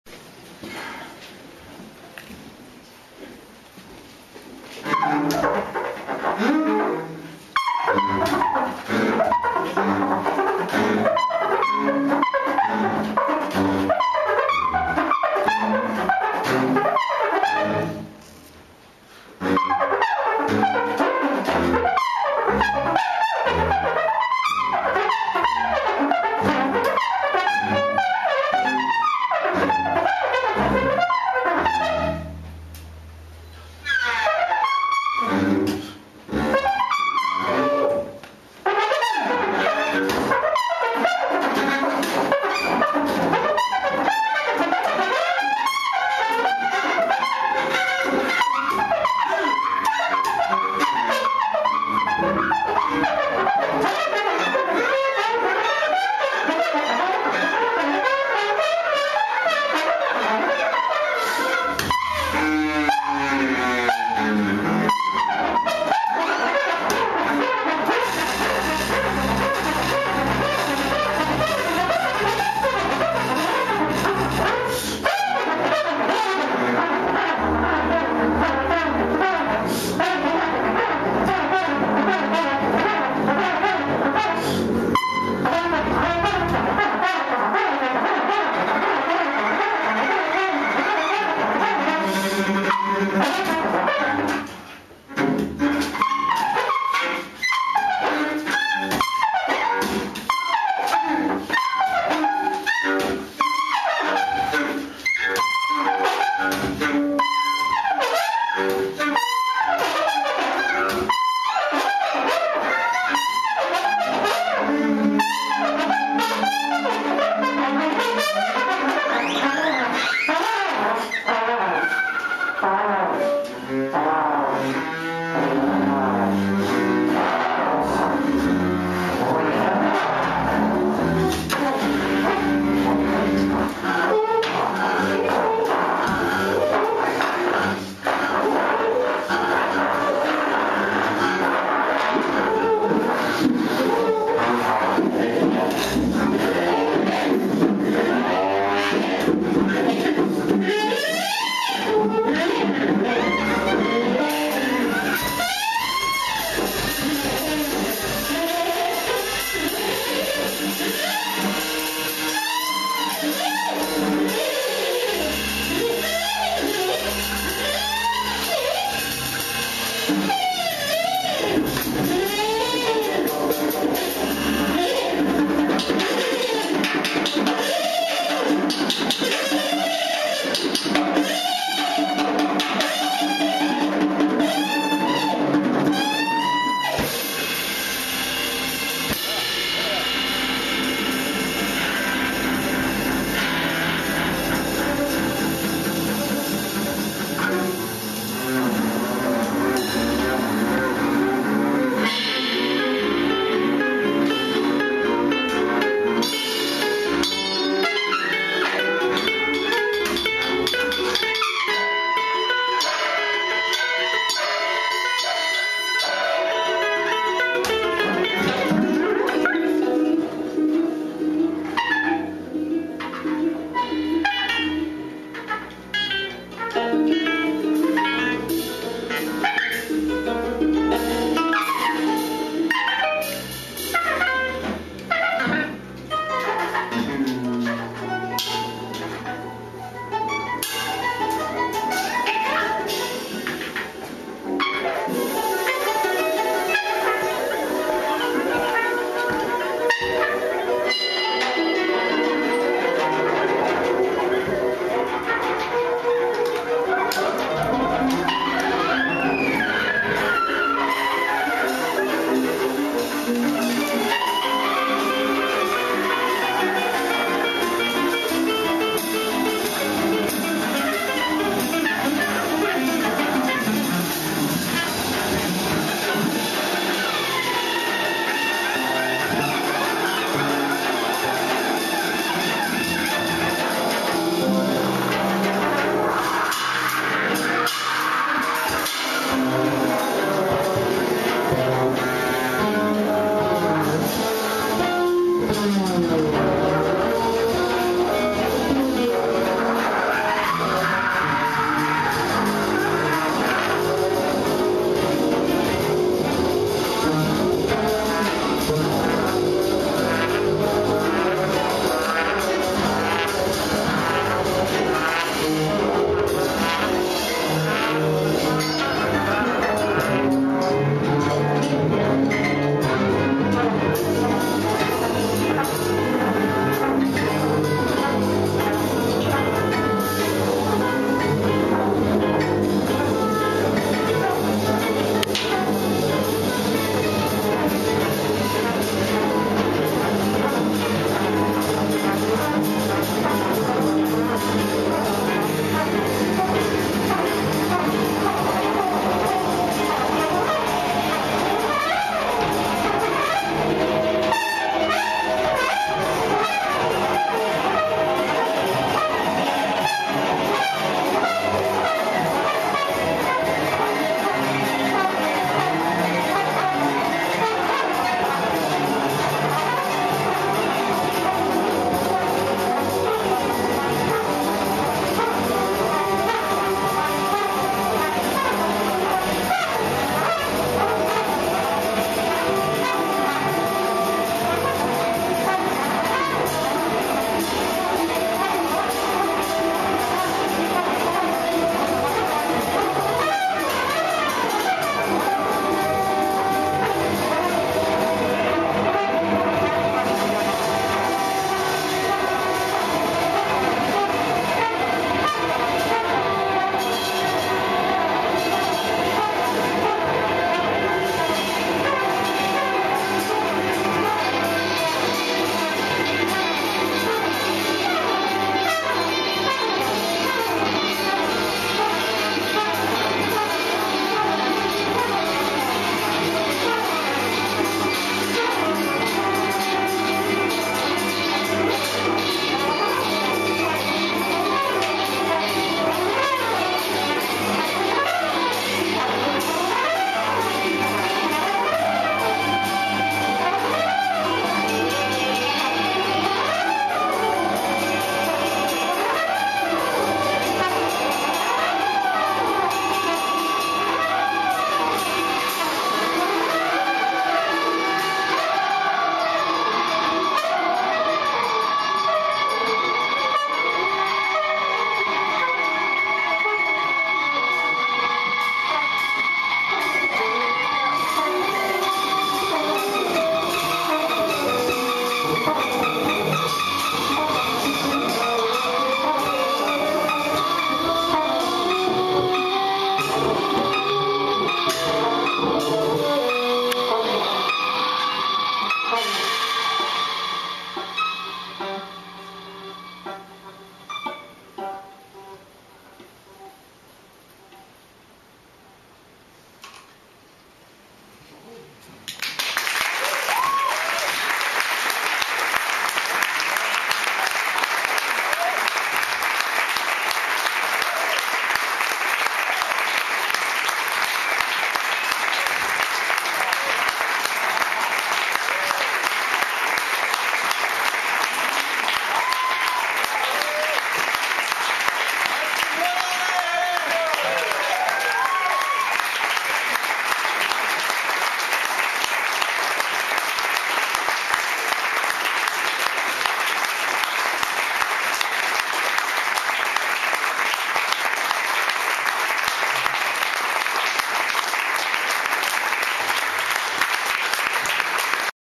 TIME FLIES FESTIVAL 2010: LAST NIGHT FINALE
A selection from KDVD 097B: Featuring: Taylor Ho Bynum, Torsten Muller,
Mary Halvorson, Dylan VanderSchyff, Fred Lonberg-Holm, Jessica Pavone,
Sara Schoenbeck and Mei Han in performance at the 22nd annual TimeFlies
Festival. A KINGDUBVIDEO PRODUCTION. COPYRIGHT: BRAD WINTER 2011.
Notes: This was the finale on the last night of 3 consecutive evenings
featuring these 8 musicians in a series of mixed improvisational
meetings (Feb. 4-6, 2011). The performances took place at The
IronWorks, Vancouver, Canada. The Festival is produced by The Coastal
Jazz and Blues Society.